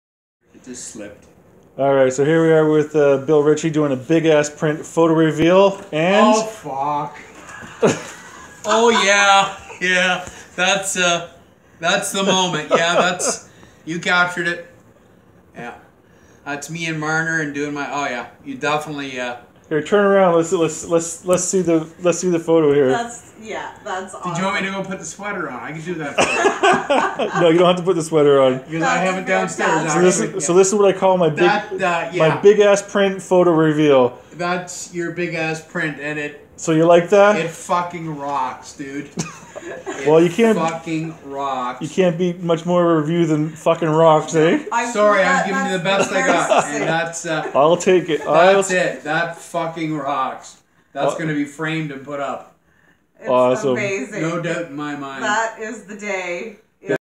Bill Ritchie/Photo Reveal
When I shoot shows, sometimes a moment stands out and I want to share it with my subject.  In this case it was a photo of Bill from his tribute show at The Corral. So a "big ass" print was made and presented to him, his reaction was priceless!